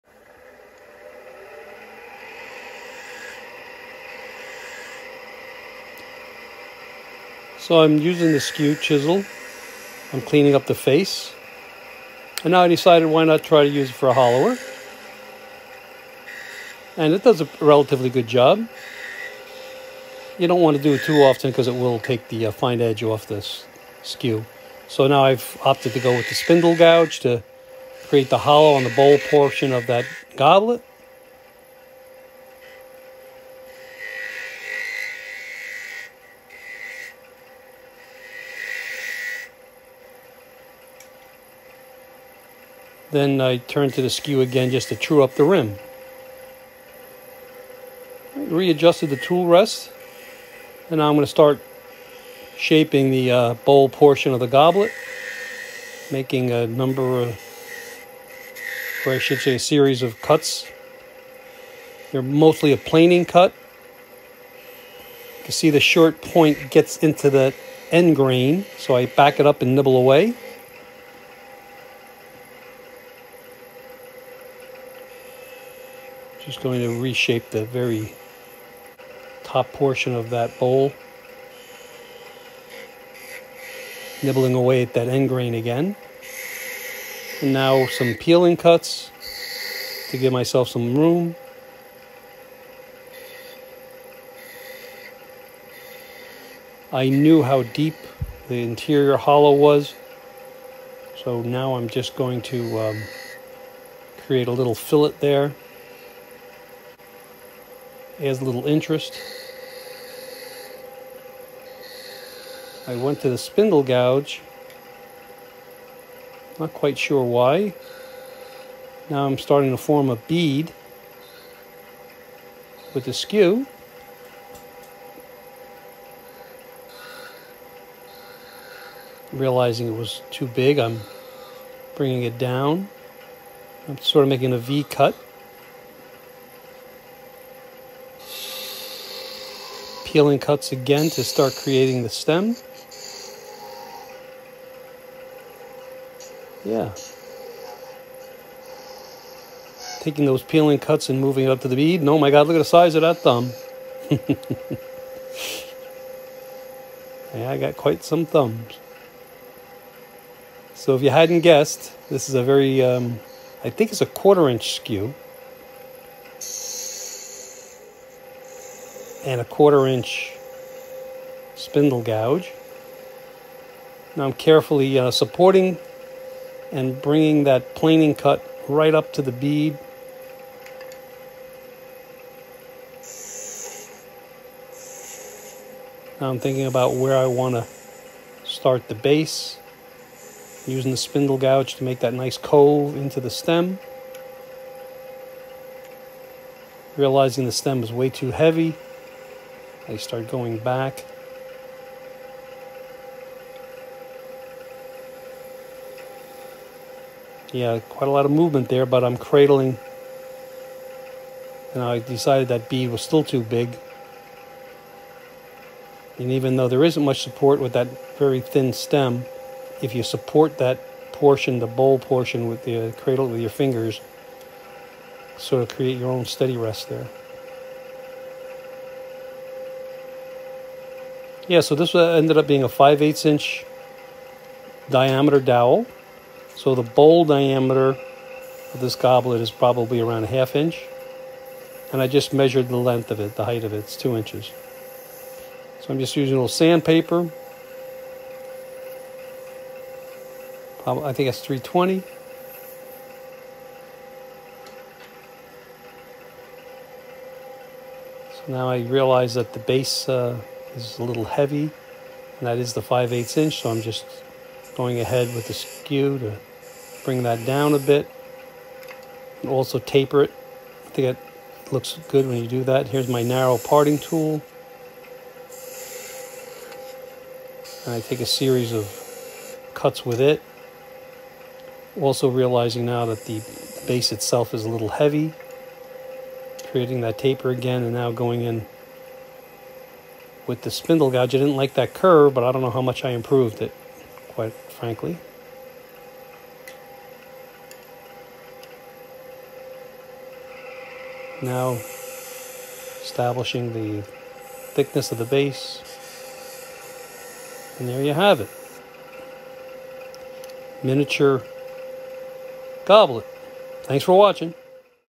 Five minute goblet
Woodturner Joe Larese makes a goblet in 5 minutes.  Stay through to the middle for a surprise!